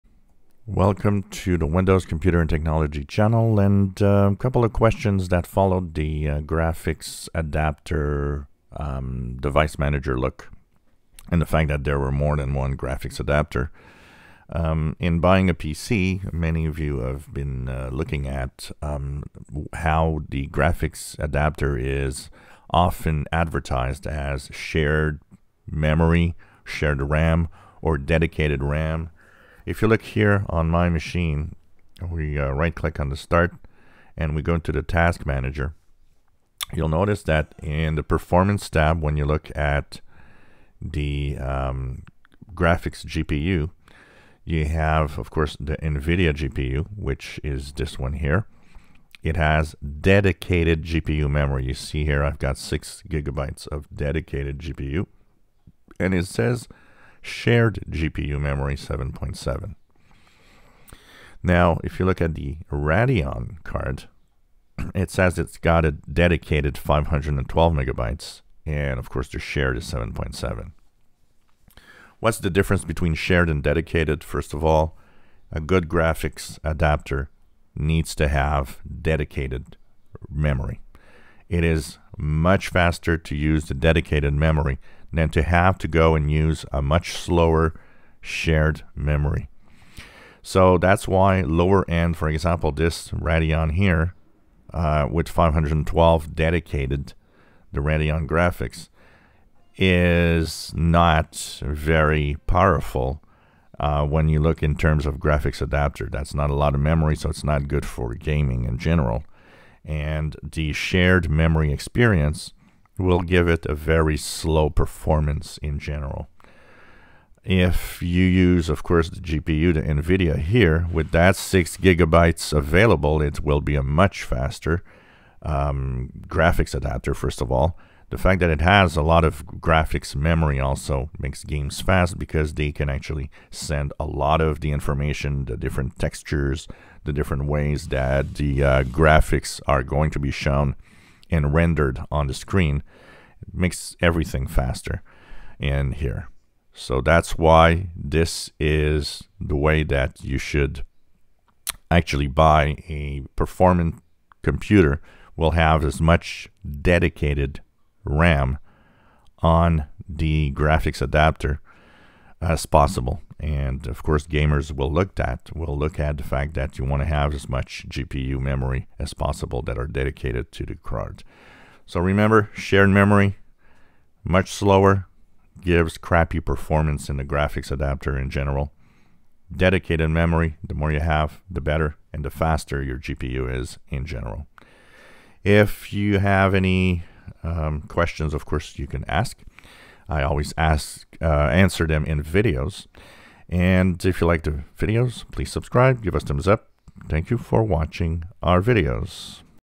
Graphics Adapter Dedicated VS Shared memory viewer question
You want a GPU that has as much Dedicated memory as possible